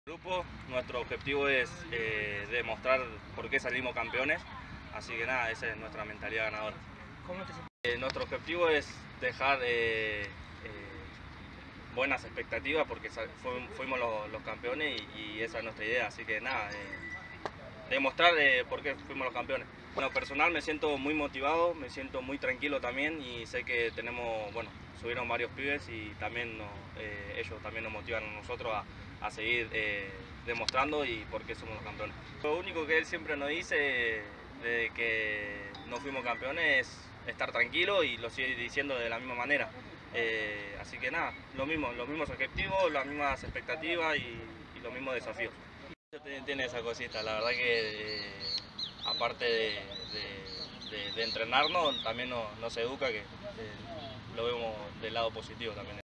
🐕🎙️ González y el objetivo de redoblar la apuesta para la Reserva
Lucas González dejó en claro que objetivos se traza la Reserva campeona de cara al 2024, mientras llevan adelante su pretemporada. ¡Esto dijo el “Galgo!